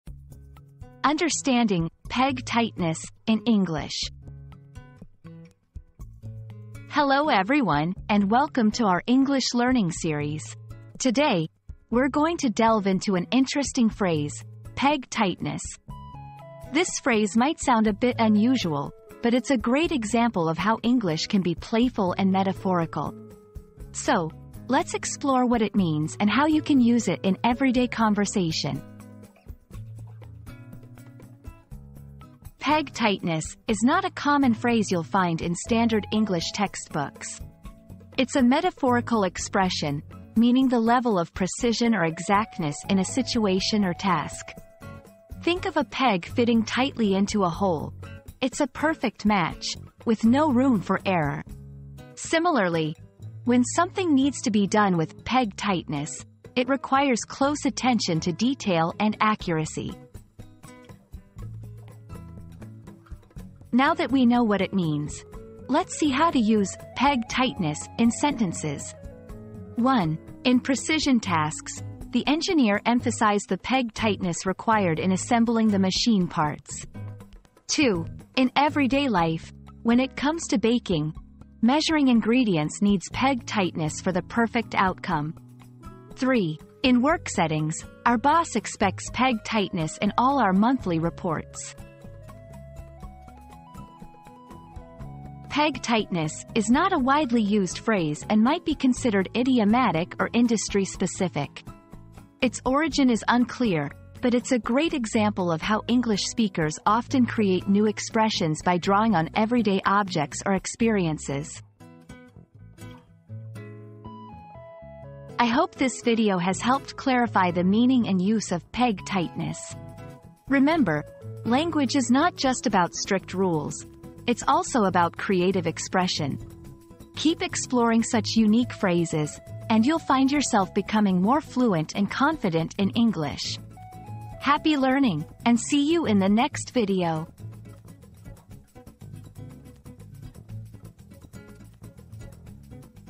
Understanding "Peg Tightness" in English
Unlocking the Secret of Peg Tightness in English • Discover the hidden meaning behind 'Peg Tightness' in English pronunciation and learn how to master this essential aspect for fluent communication.

00:00 • Introduction - Understanding "Peg Tightness" in English
00:32 • What Does "Peg Tightness" Mean?
01:03 • Using "Peg Tightness" in Sentences
01:34 • The Origin and Rarity of the Phrase